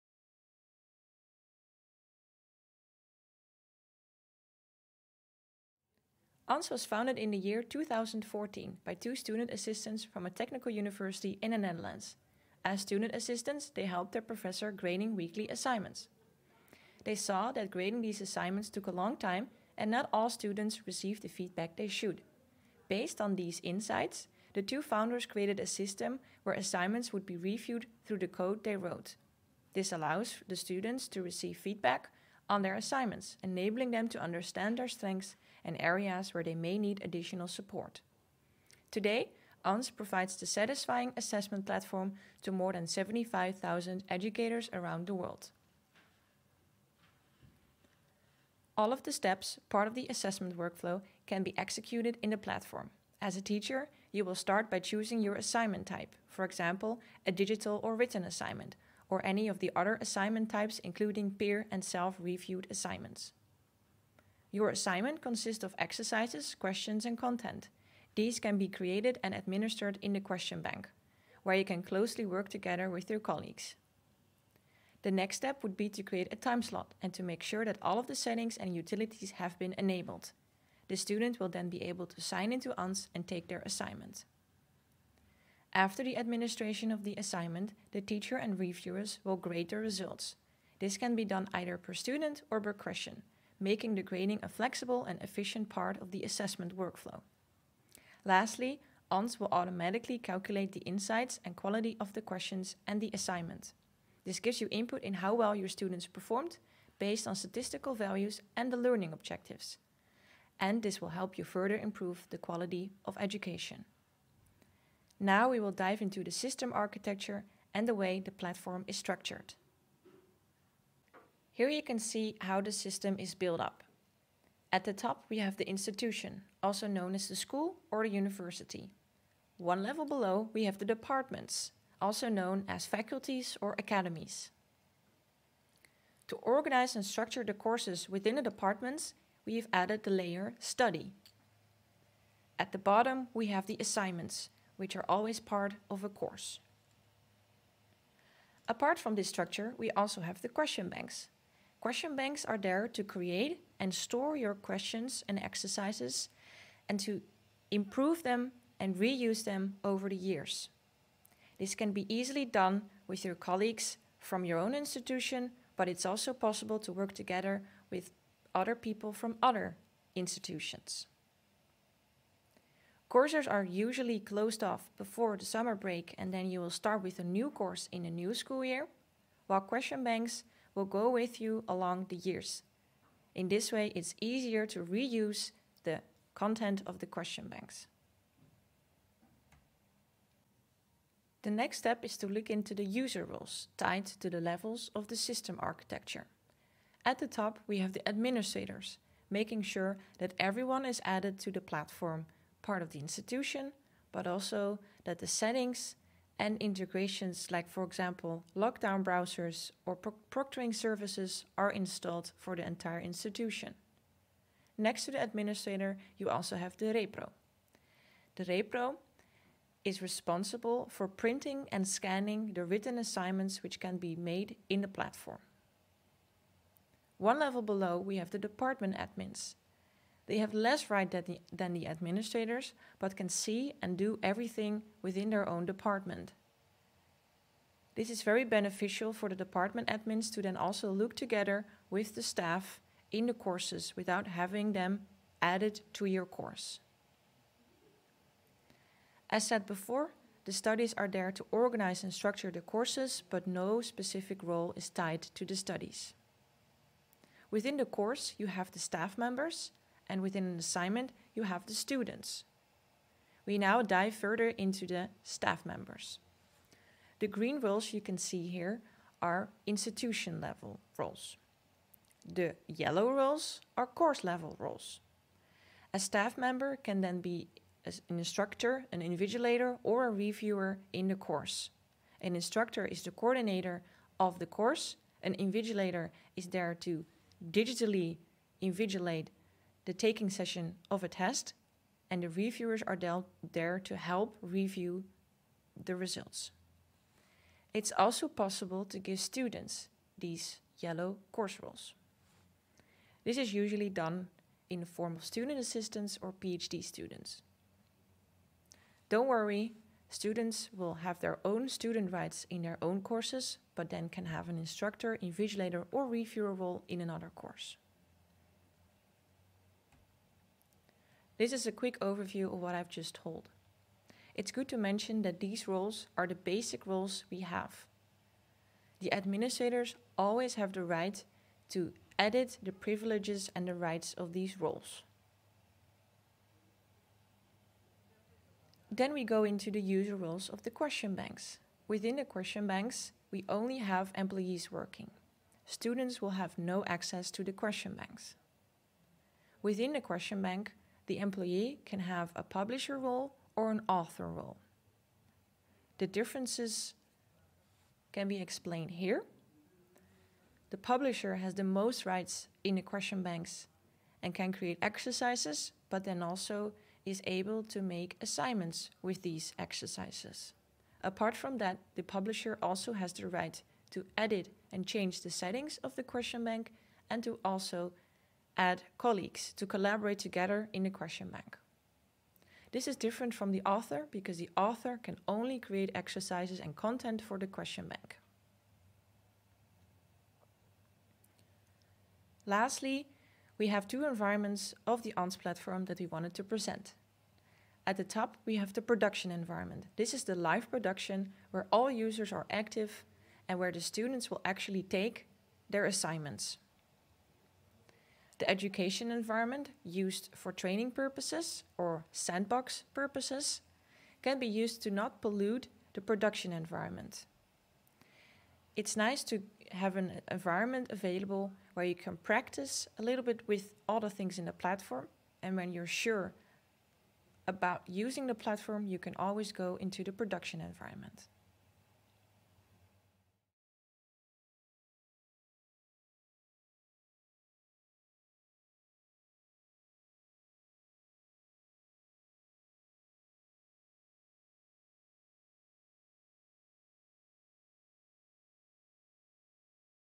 Basic Training | Part I: Introduction
Welcome to the first video of the Ans basic training!

The basic training teaches the overall exam process, from designing an assignment blueprint to improving a question bank based on assignment analyses. All basic functionalities of the platform will be discussed during this training. The training series consist of 6 separate videos.
In the first training video we will briefly introduce Ans. After that we will explain more about the system architecture and the user roles. Last, we introduce the different environments of Ans before we navigate to the platform.